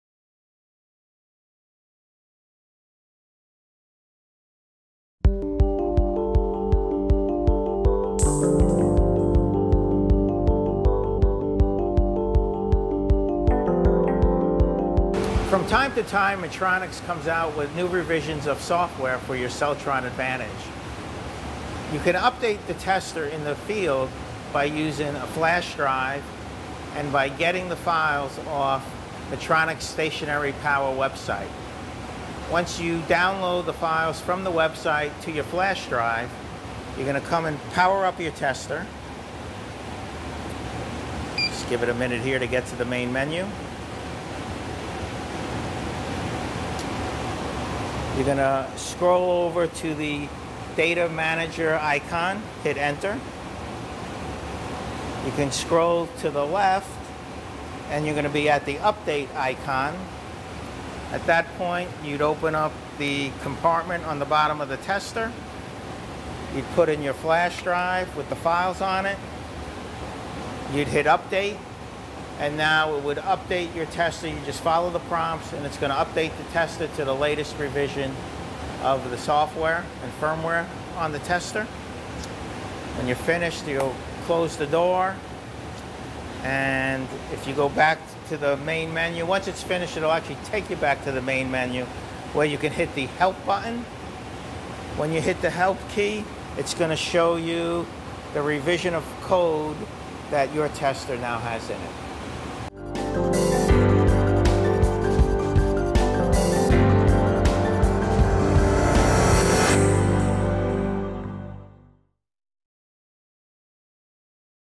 Updating The Midtronics Celltron Advantage Battery Analyzer
Ed Amato, Technical Training Manager and IEEE Committee Member explains, step by step, how to update the Midtronics Celltron Advantage Battery Analyzer.  For use in Stationary Power, Critical Power applications.